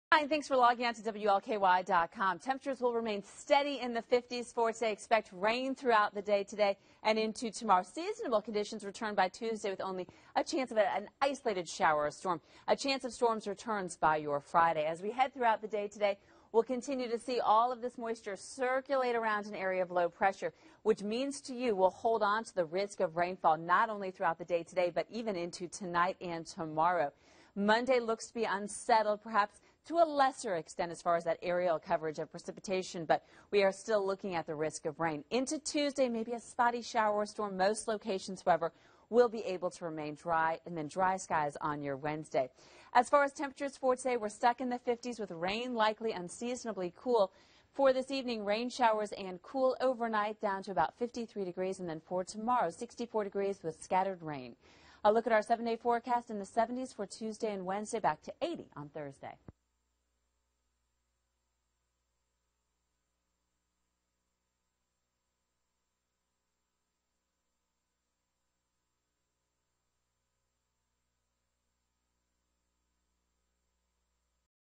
Sunday Weather Forecast
Rain continues to impact the WLKY viewing area. Susanne Horgan has a look at your back to work weather.